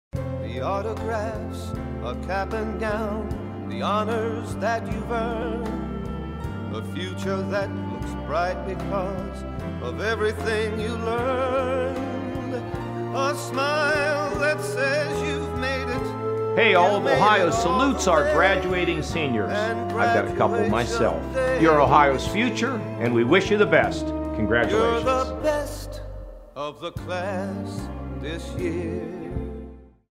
Ohio Lottery Best of the Class 2018
Ohio Lottery Best of the Class featuring students from Austintown Fitch, Reynolds, Wilmington Area.